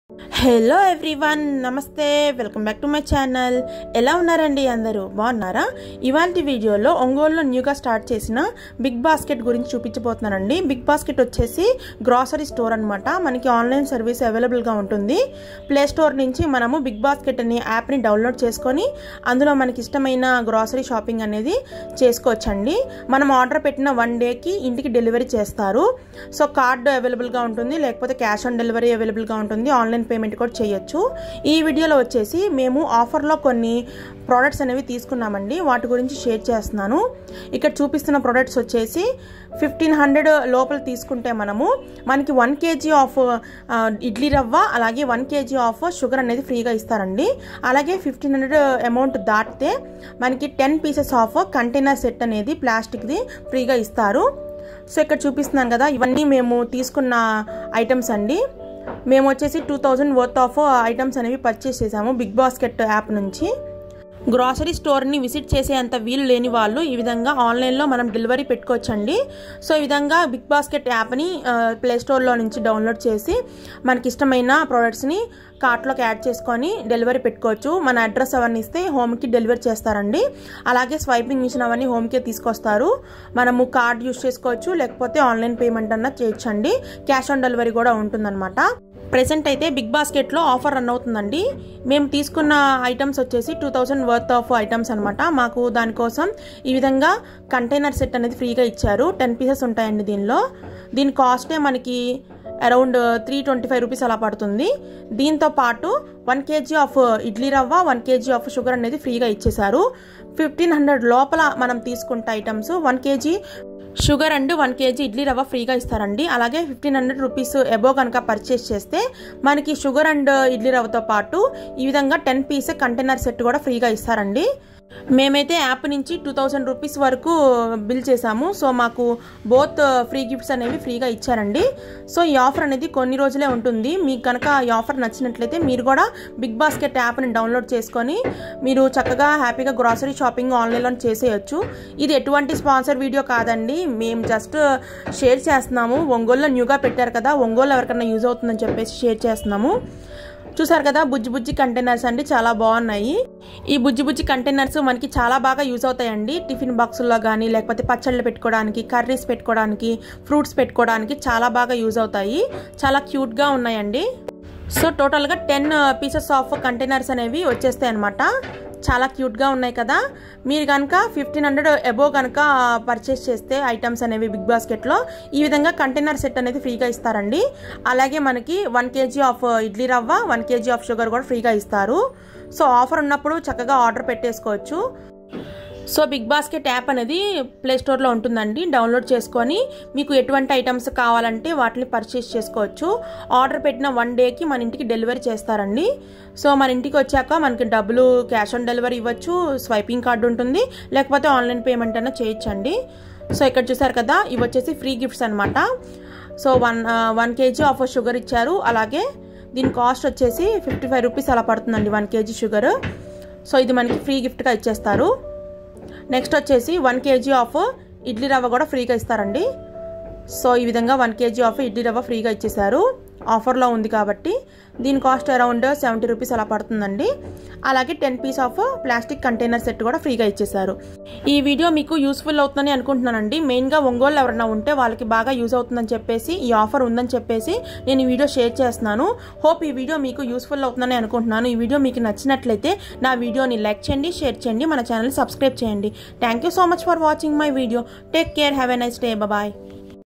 Bigbasket Offer In Ongole | Ongole Bigbasket Grocery Store | Get Free Gifts On Grocery Purchase
Hi guys,

In this video I shared Bigbasket Offers In Ongole. Buy 1500rs/- & above worth products from Bigbasket then get free gifts.

Bigbasket In Ongole